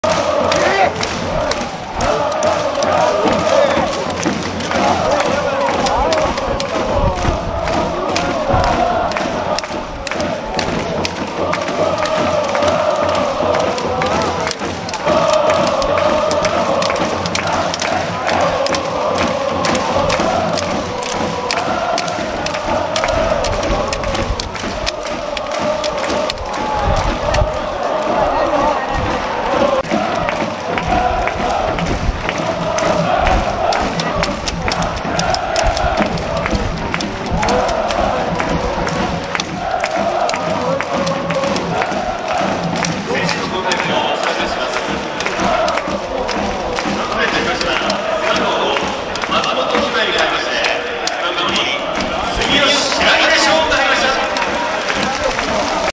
【後半38分】大弥 から ジェラ へ選手交代⚽⚽ 2023.03.26 YBCルヴァン杯 グループステージ第2節 名古屋グランパス 戦
日本郵便 スポンサードゲーム